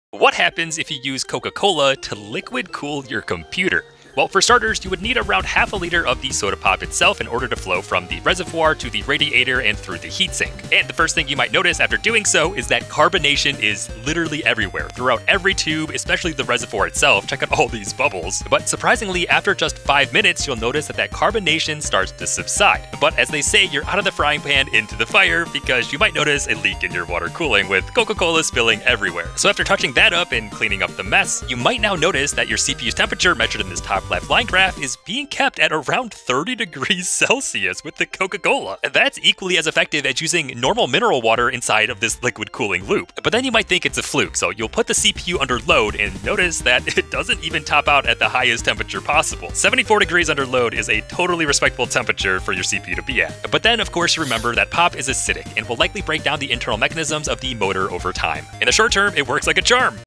this Coca Cola cooled PC is wild
this Coca Cola cooled PC is down right ridiculous..but it works surprisingly well in the short term! wouldn't count on it holding up over time though 😅